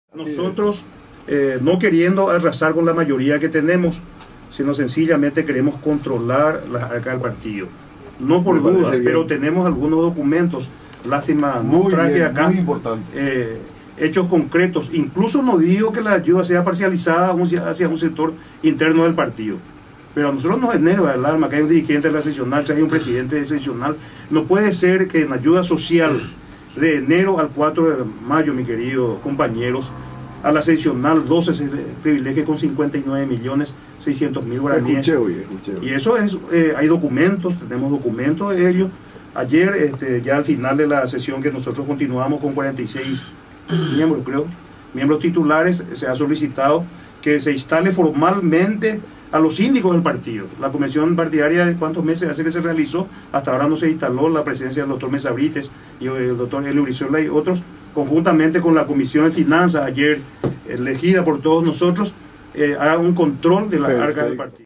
"Queremos controlar las arcas del Partido"
"No puede ser que de enero a mayo, a la seccional 12, se le privilegié con 59 millones de guaranies" reclamó Angel Pintos Balbuena, con respecto al destino dado a la ayuda social colorada, durante la gestión de Lilian Samaniego.

Pintos Balbuena habló, en la 103.7 FM radio Exclusiva, de "hechos concretos" y de la existencia de "documentos" que demuestran un manejo desprolijo de los activos de la ANR.

"Se ha solicitado a los síndicos del Partido, conjuntamente con la comisión de finanzas, para que hagan un control de las arcas del Partido", concluyó.